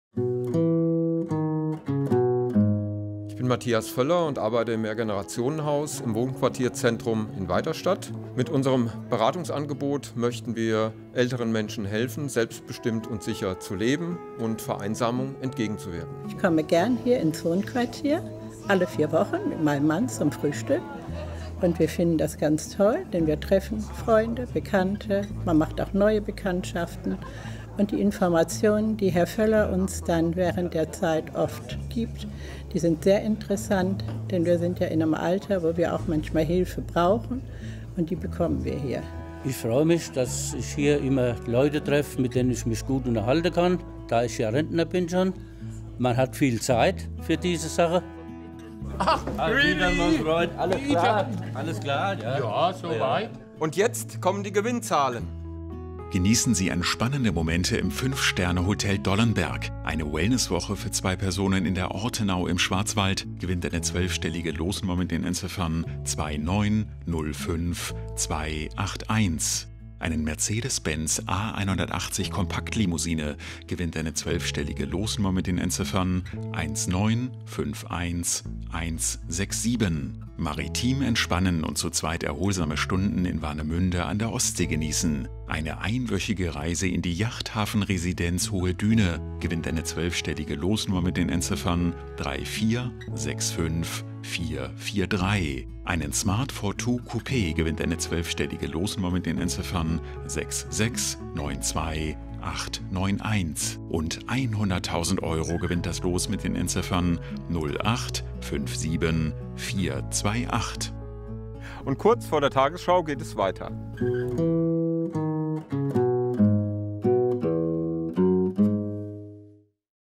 Wochenziehung, 13.01.2019 (Weiterstadt)
Die Gewinnzahlen der Wochenziehung präsentiert Ihnen Matthias Völler, Sozialarbeiter im Mehrgenerationenhaus Weiterstadt in Hessen. Fördersumme: 100.000 Euro